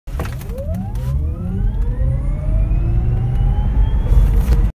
Tesla Model S 85D 0-60 Run
-right on track for rated 4.2 seconds
-half charge
-driver and passenger (300 lbs total)